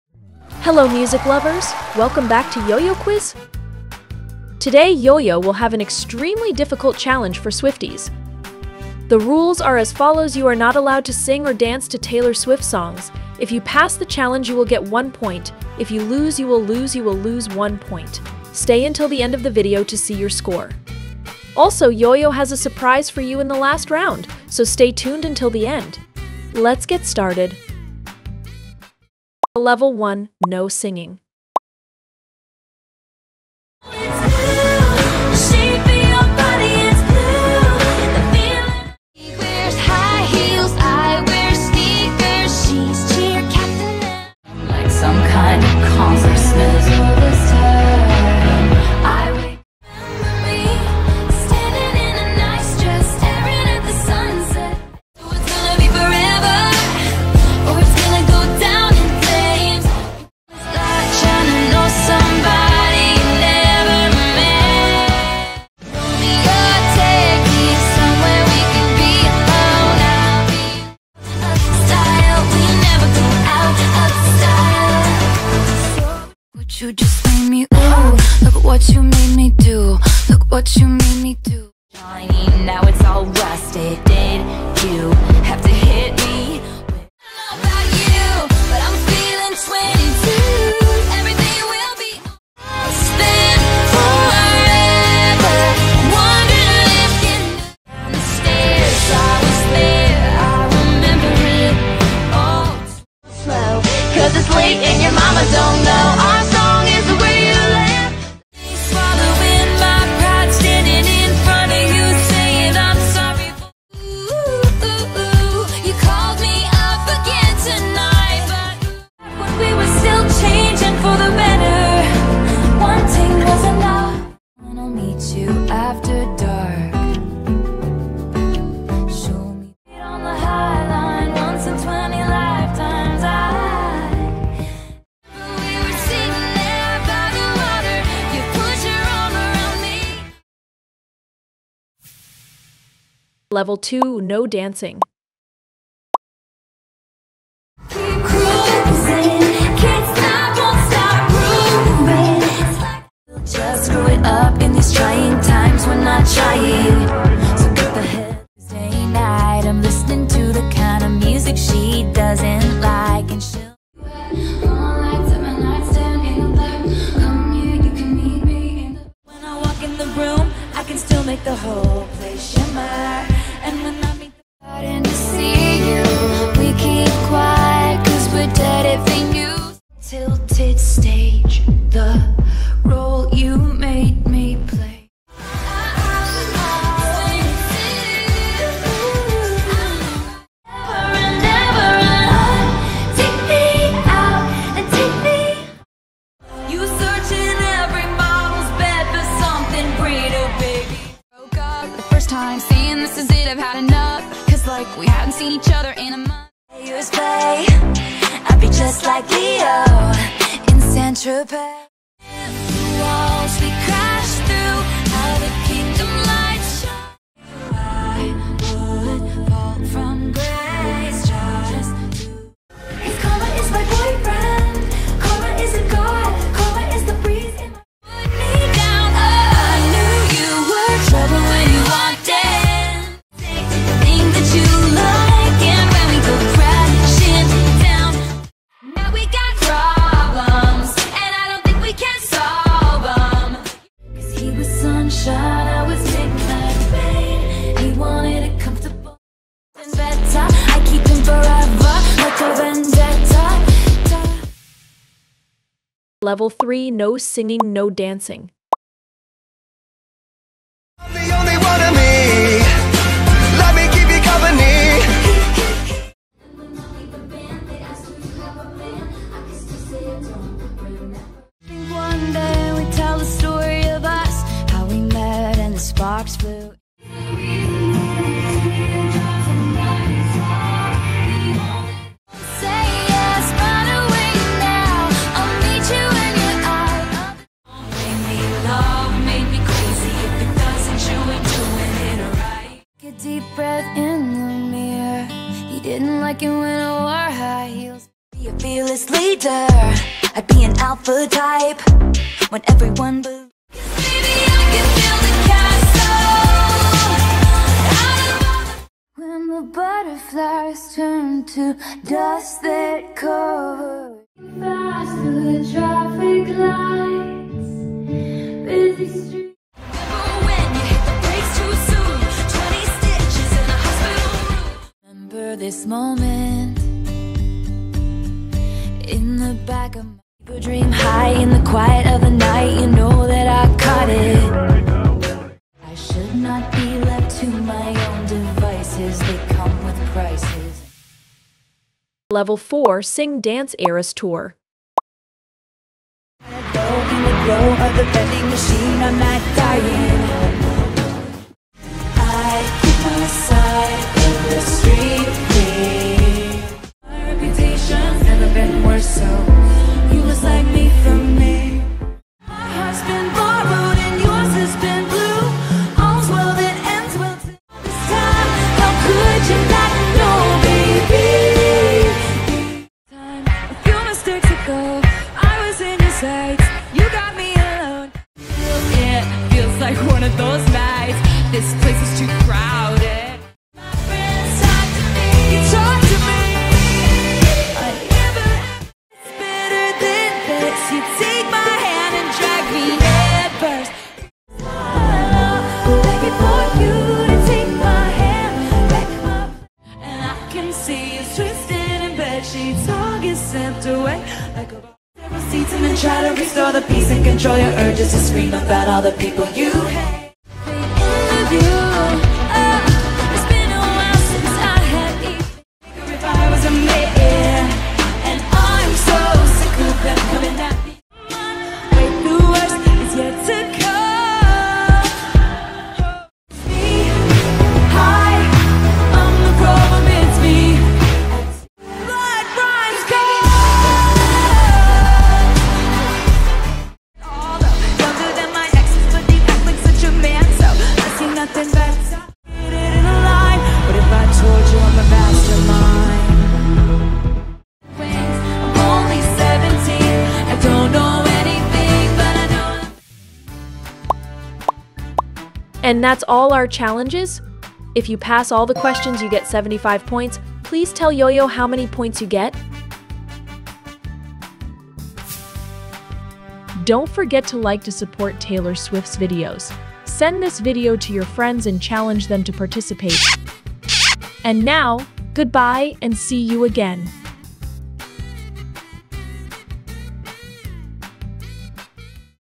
Try Not To Sing or Dance🎶Taylor Swift Edition 😘⚠ Only for Real Swifties🎸📝
Try Not To Sing or Dance🎶Taylor Swift Edition 😘⚠ Only for Real Swifties🎸📝Test your Taylor Swift knowledge and self-control in this Try Not To Sing or Dance Challenge! Only for real Swifties who think they can resist jamming out to Taylor's hits. Let's see if you can keep a straight face while listening to these catchy tunes!